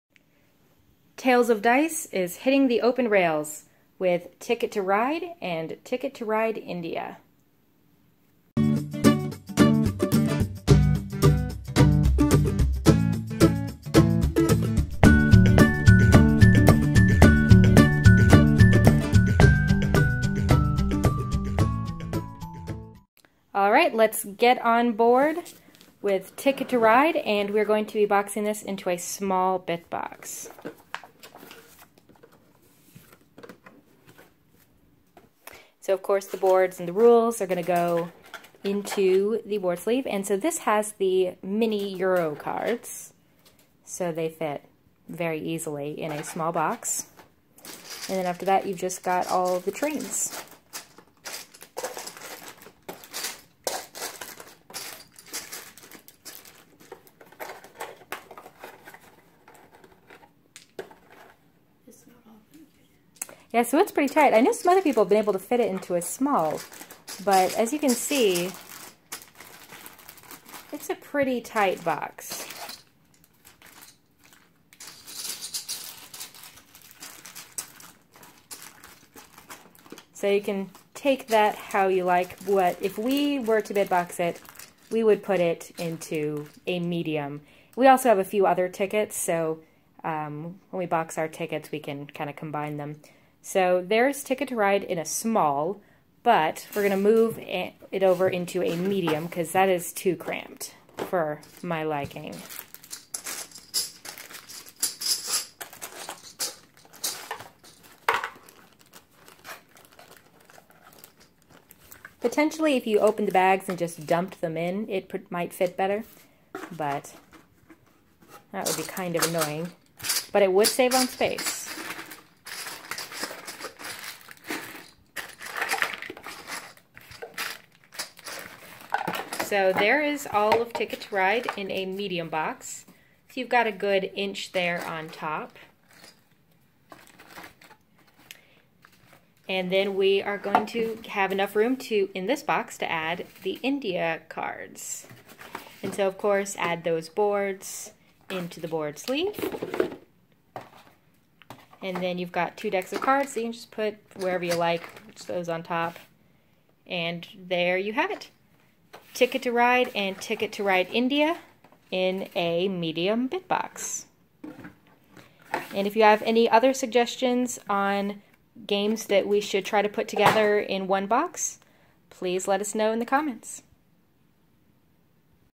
Ticket to Ride and India Expansion BITBOX Test
BITBOX testing Ticket to Ride and the India and Switzerland expansion.